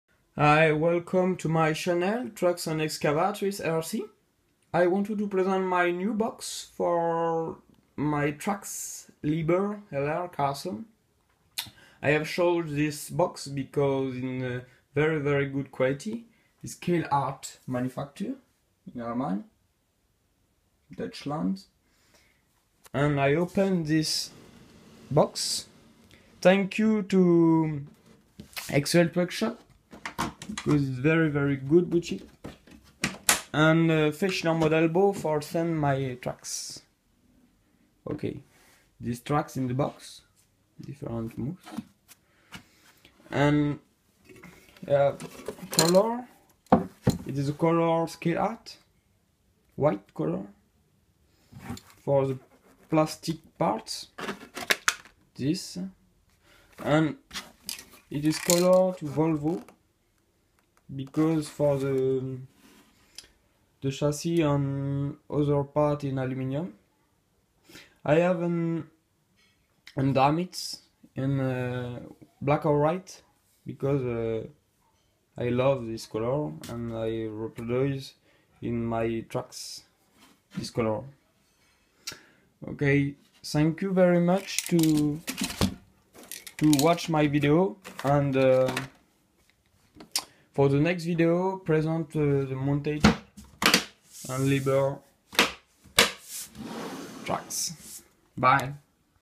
liebherr lr634 carson rc 1/14 scaleart box
i present in my vidéo
my lr634 carson trax liebherr and this box scaleart
it is an very good quality and good style
for the next vidéos an differents part in kit carson laderaupe

excuse my for my english because i am an french boy